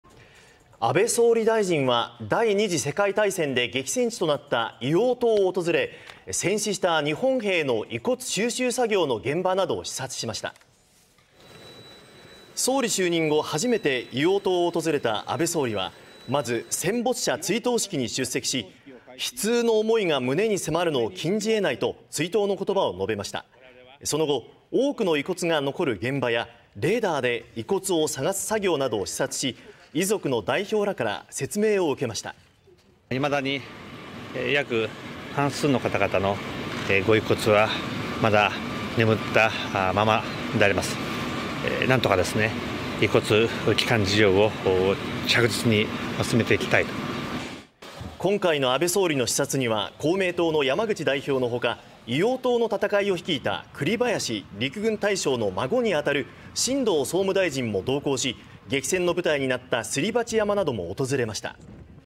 安倍総理が硫黄島訪問 日本兵の遺骨収集を視察(13/04/14)
安倍総理大臣は、第2次世界大戦で激戦地となった硫黄島を訪れ、戦死した日本兵の遺骨収集作業の現場などを視察しました。